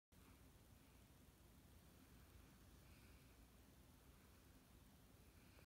Question #518
Oh yes it is way better than the Nutcracker and the four realms because Ralph breaks the Internet Beat the living crap out of it at the box officeAnd I can’t believe the Nutcracker and the four realms comes out on Blu-ray on January 29 which I am not getting it instead Ralph breaks the Internet made a lot of money in this movie Because Yesss is way better than sugarplum.